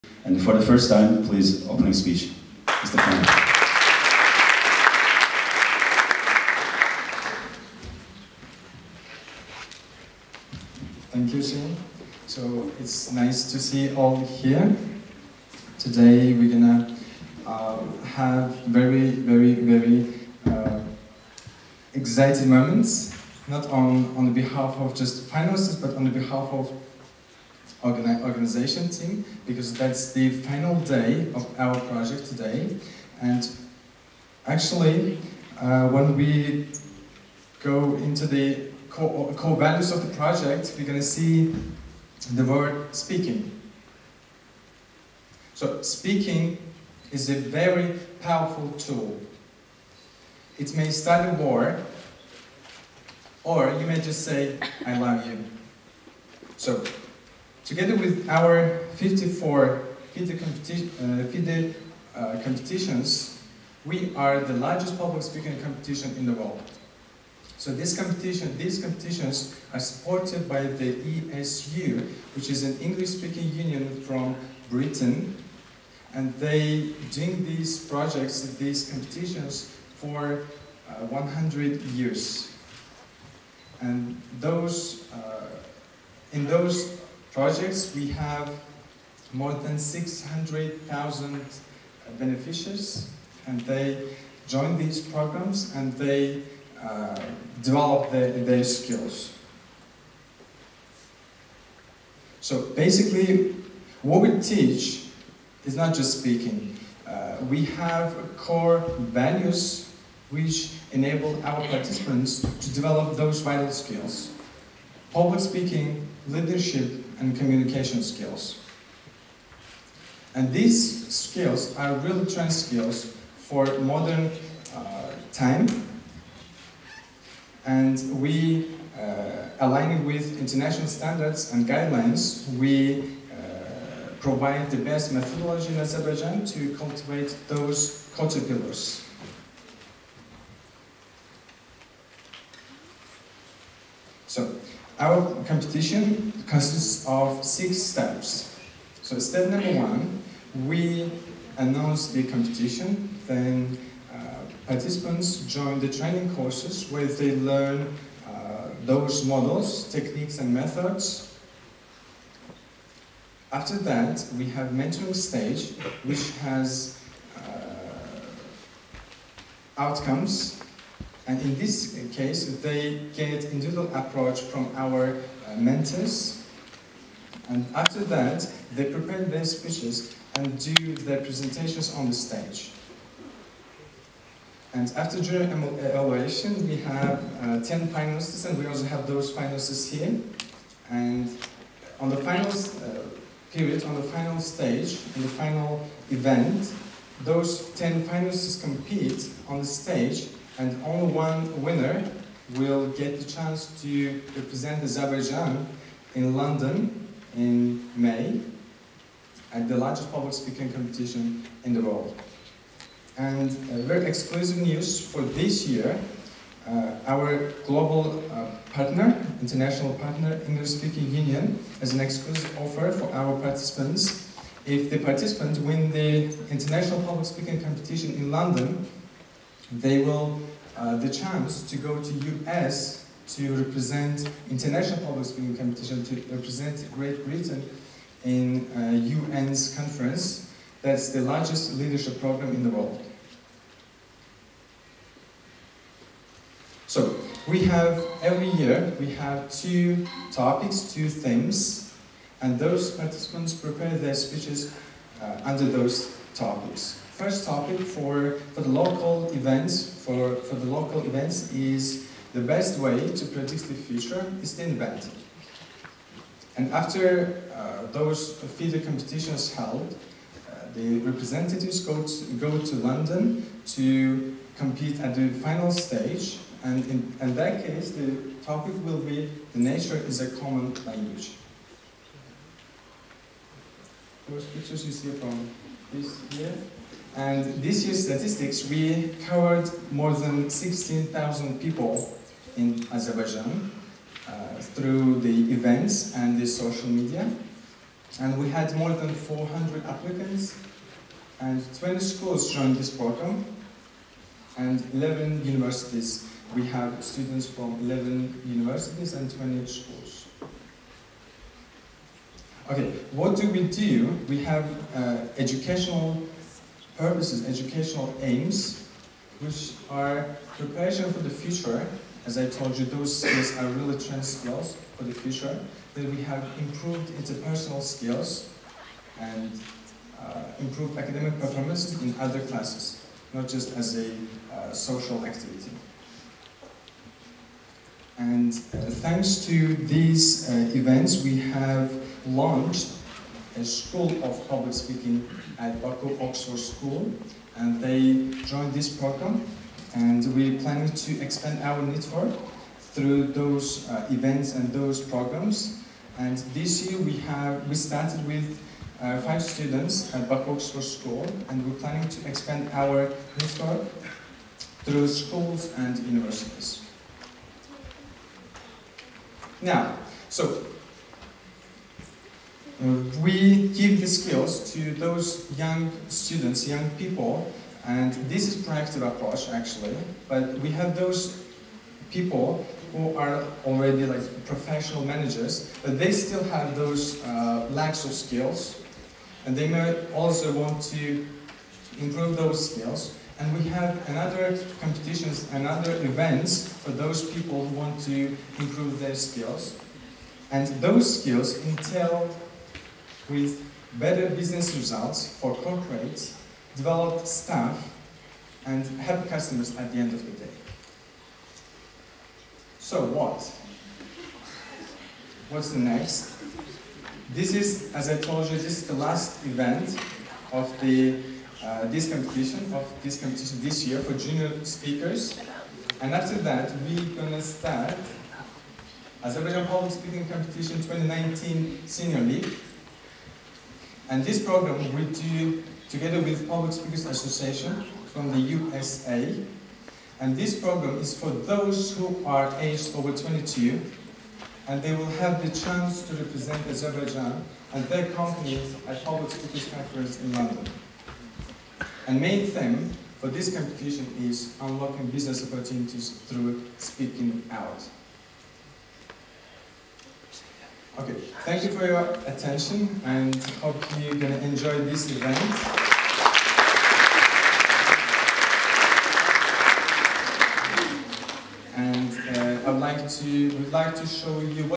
Kenan MAMEDOV - Creating The Climate For Cultivating Caterpillars
Azerbaijan Public Speaking Competition - Project Presentation.